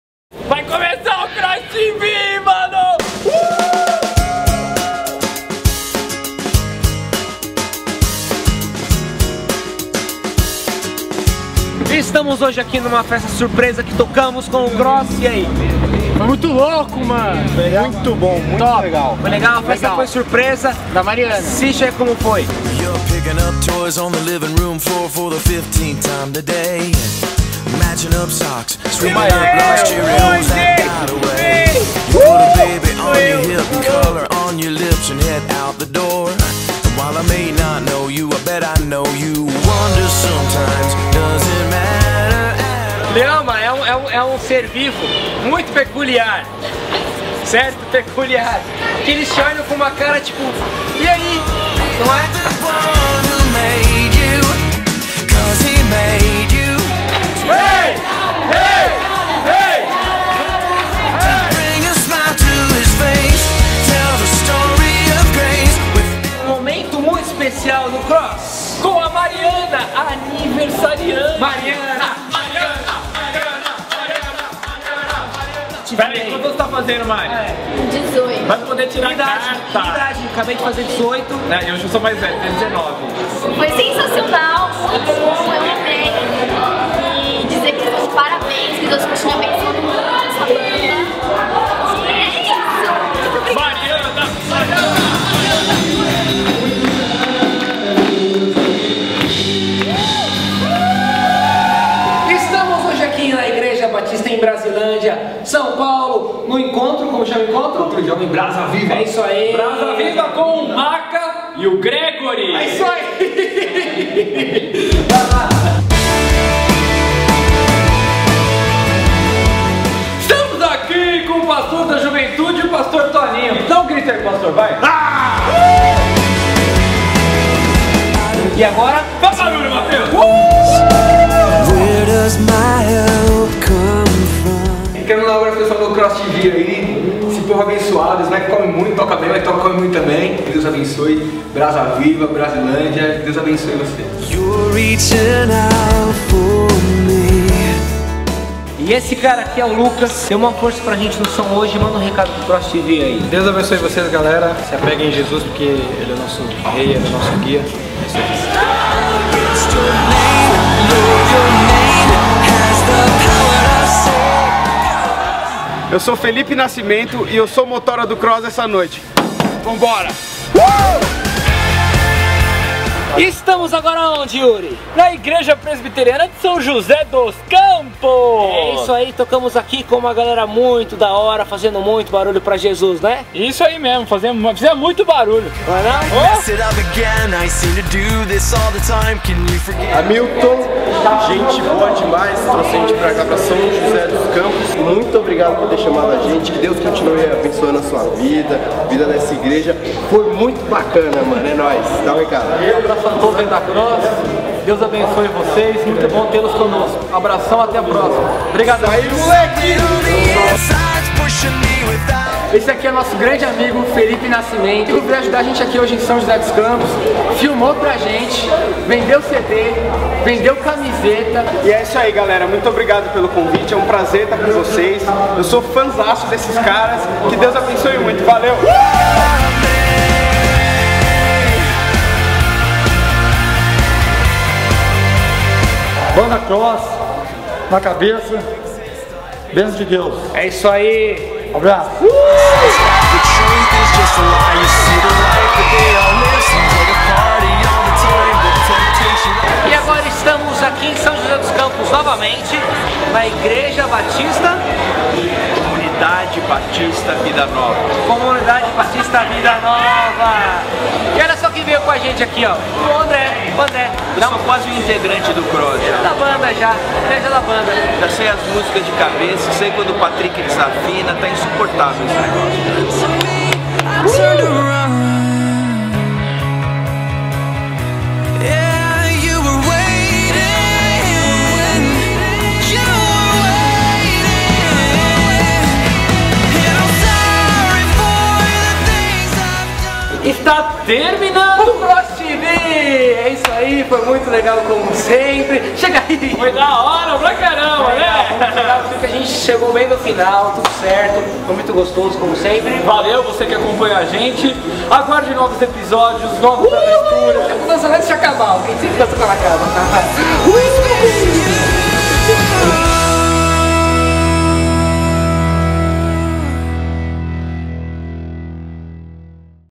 Cross TV [Episódio #43]
E novamente um episódio com muitas pessoas especiais em lugares especiais!! Aniversário de Mariana, Igreja Batista em Brasilândia-SP, Igreja Presbiteriana Central de São José dos Campos-SP e Comunidade Batista Vida Nova-SJC. VALEU DEUS!!! Com Patrick Peticov, Yuri Danka, Pedro Duduch e Gibimba Dias